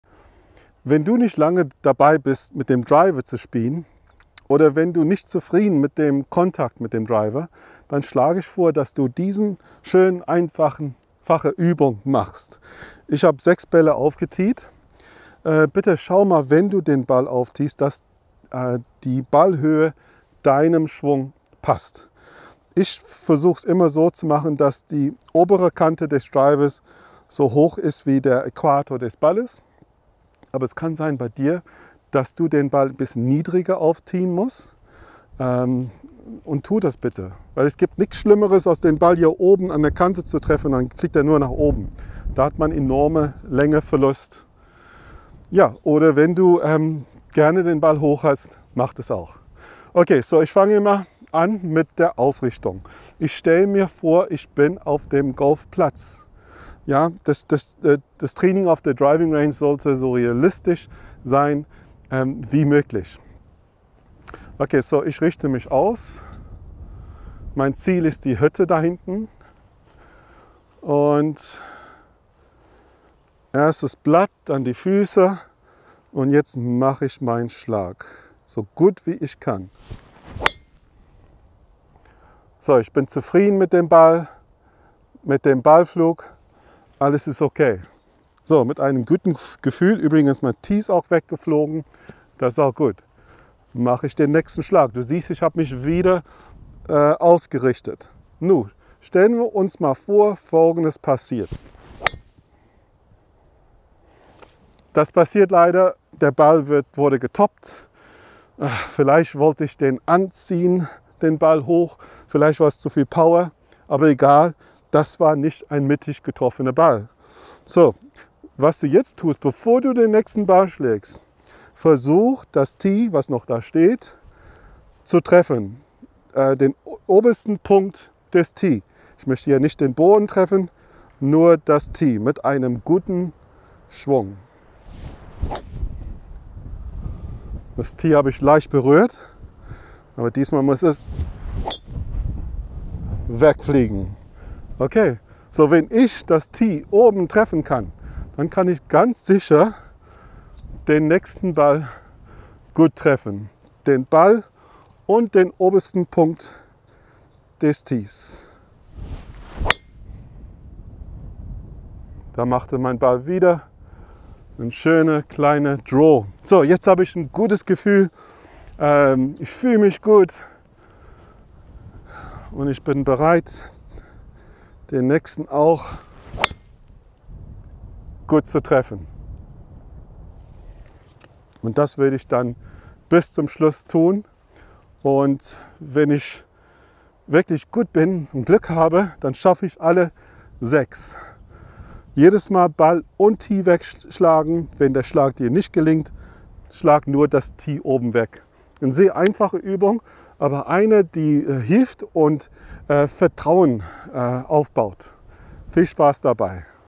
Diese Driver-Übung (für Golf-Neulinge) macht Spaß
Um besser mit dem Driver spielen zu können, ist es wichtig, dass wir den Ball mit dem Sweetspot des Drivers treffen. Diese kleine Übung wird dir definitiv helfen, das zu tun!